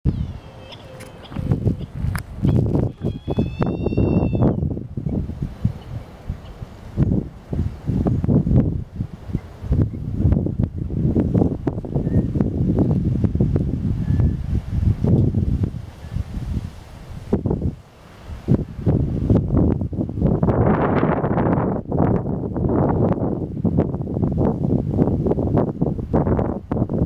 Kermadec Petrel pair- with vocalization
Kilauea Point National Wildlife Refuge
Kaua'i, Hawai'i, USA
29 August 2018